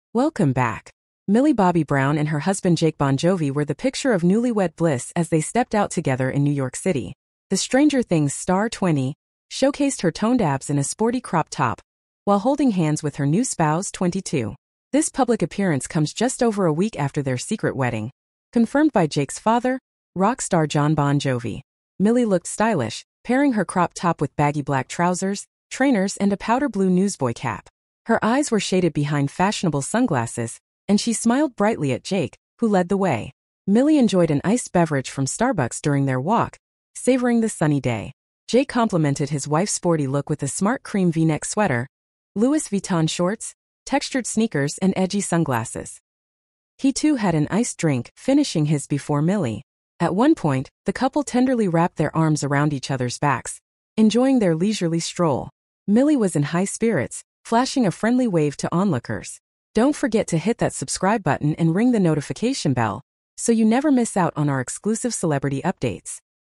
Millie Bobby Brown & Jake Bongiovi step out together in NYC, After their secret wedding
Millie Bobby Brown and her husband Jake Bongiovi were the picture of newlywed bliss as they stepped out together in New York City. The "Stranger Things" star, 20, showcased her toned abs in a sporty crop top, while holding hands with her new spouse, 22. This public appearance comes just over a week after their secret wedding, confirmed by Jake's father, rock star Jon Bon Jovi.